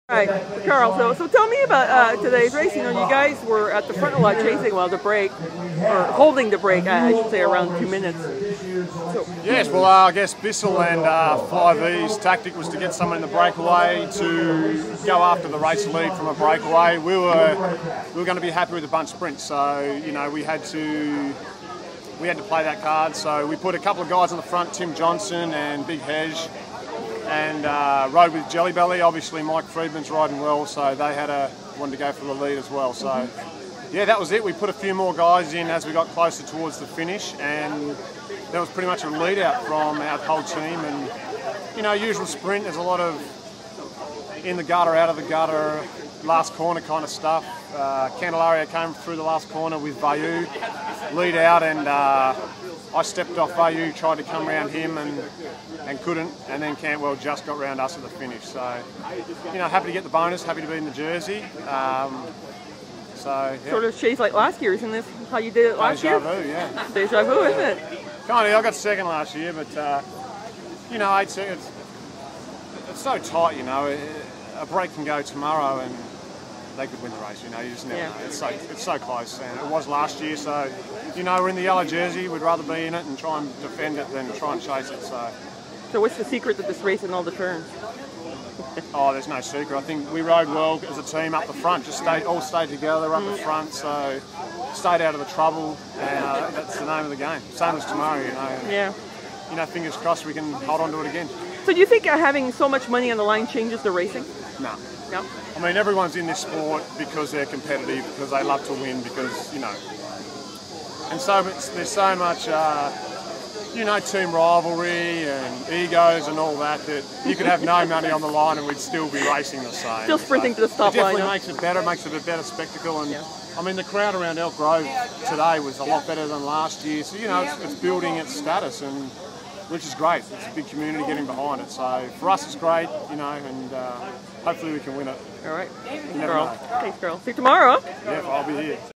Tour of Elk Grove Stage 2 with Karl Menzies
Post-stage chat with Karl Menzies (UnitedHealthcare p/b Maxxis). Menzies finished third on the stage and became the new leader after two stages at the Alexian Brothers Tour of Elk Grove.